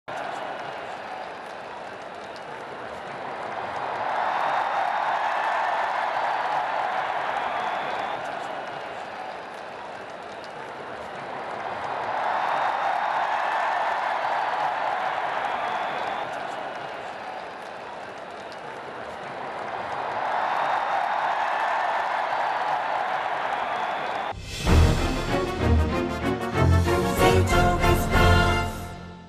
अहमदाबाद | भव्य समारंभासाठी गुजरातच्या जनतेचं अभिनंदन - मोदी
Ahmedabad PM Modi Greets And Hug US President Donald Trump